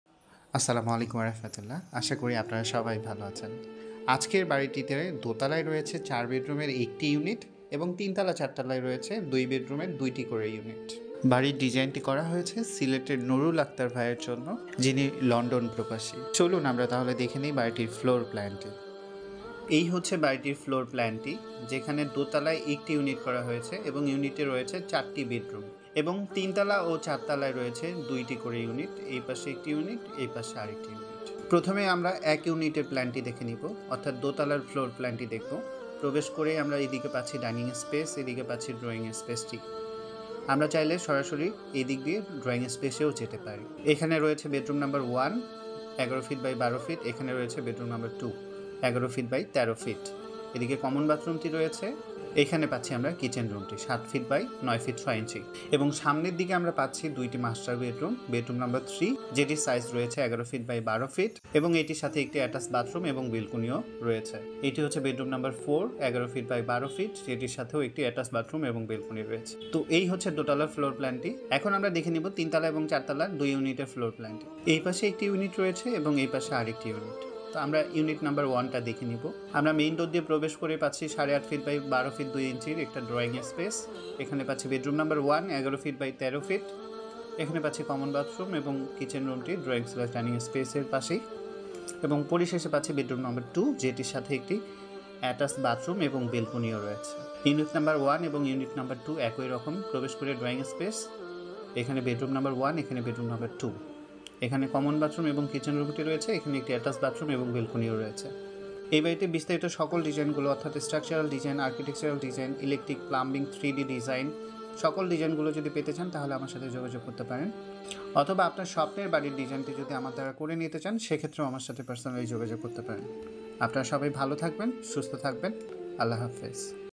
2 Unit House Design in Bangladesh | 4 Bedroom House Design Bangladesh
দুইতলায় চার বেডরুমের একটি ইউনিট এবং তিনতলা ও চারতলায় রয়েছে দুইটি করে ইউনিট।
আপনার স্বপ্নের বাড়ির ডিজাইনটি আমার দ্বারা করে নিতে চাইলে আমার সাথে যোগাযোগ করতে পারেন ।
Md. Asikur Rahman
B.Sc in Civil Engineer (Structure)